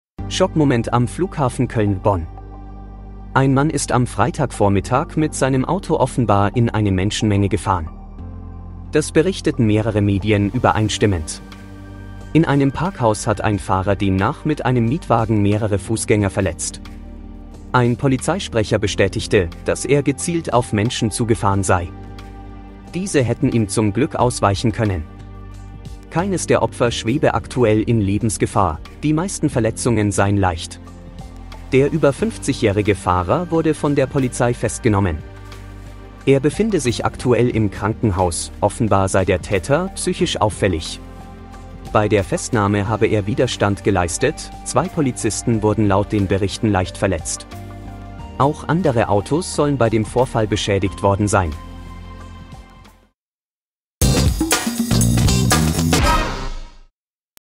Flughafen Köln/Bonn: Mann fährt absichtlich mehrere Menschen an
In einem Parkhaus am Flughafen Köln/Bonn ist ein Mann absichtlich in eine Menschenmenge gefahren. Zum Glück wurde niemand schwer verletzt. Der Täter wurde festgenommen.